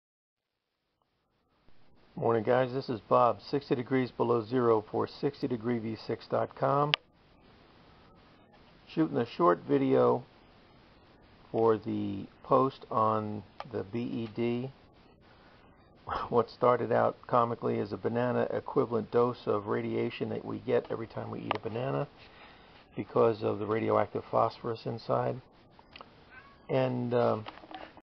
The "BED" Banana Equivalant Dose (3)
Oddly enough. There is a Measurement of Radioactivity called a "BED" or "A Banana Equivalent Dose" which is the average amount of radiation that gets ingested from eating a Banana. I wanted to find out if this was true... so I picked up a 60+ year old Geiger Counter from eBay for under $40 and by God... the thing works and the story is TRUE!. Apparently... naturally radioactive Phosphorous is taken into the Banana Plant during its growth and gets deposited in the fruit.